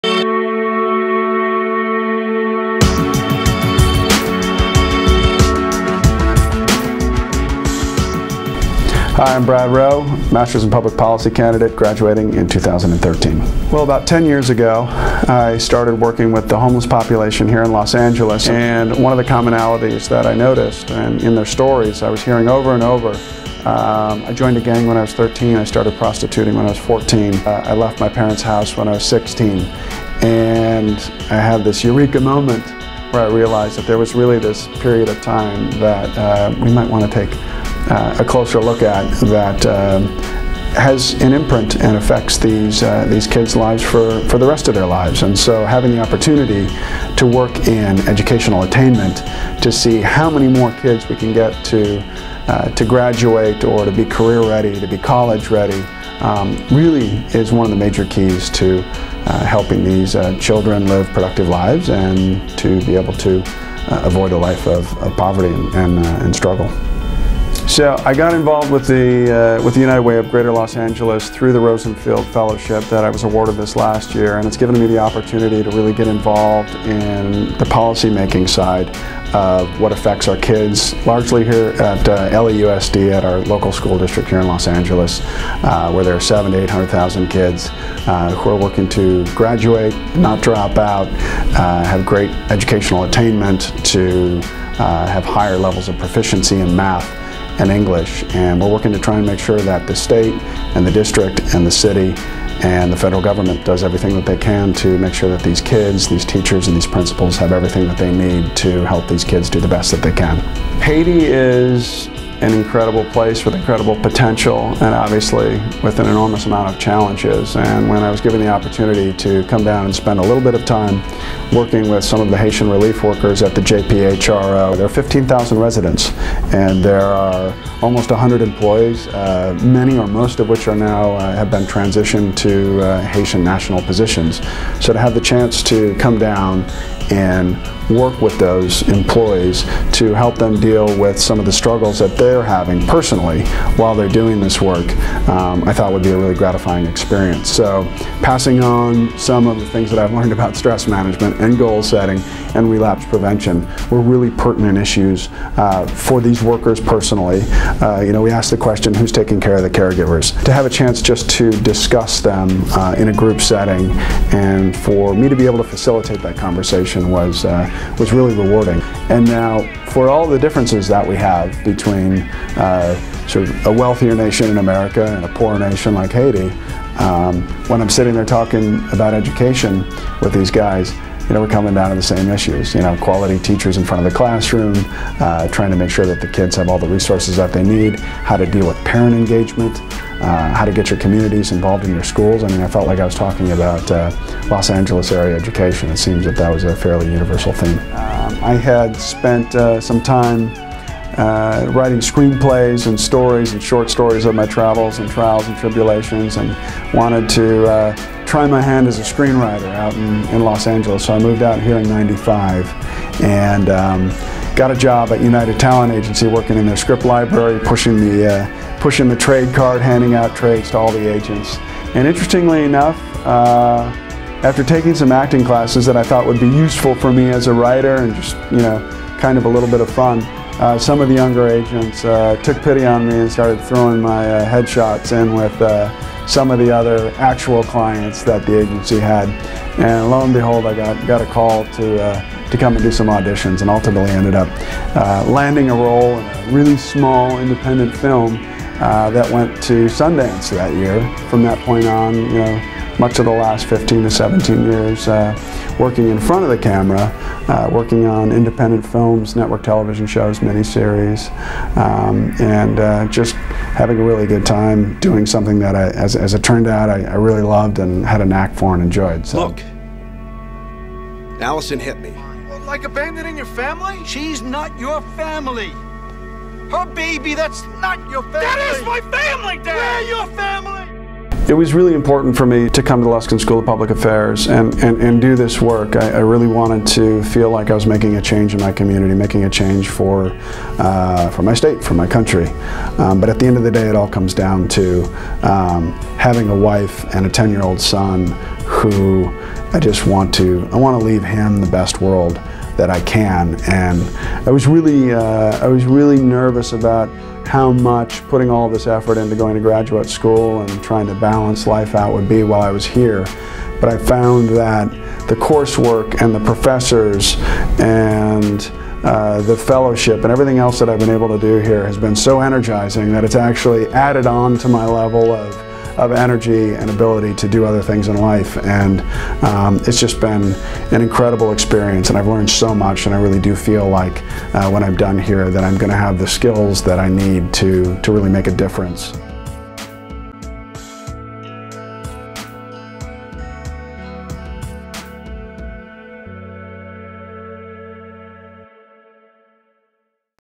Leaders at Luskin: Brad Rowe
An actor and an activist, Brad Rowe is a Public Policy student who has received a Rosenfield Fellowship and is working at United Way Los Angeles. Rowe, an accomplished actor who has been in over 70 film and television titles, traveled to Haiti to work with the J/P Haitian Relief Organization in 2013. He has also directed and produce documentaries that have featured Bill Clinton, Condeleezza Rice, Bill Gates and many others.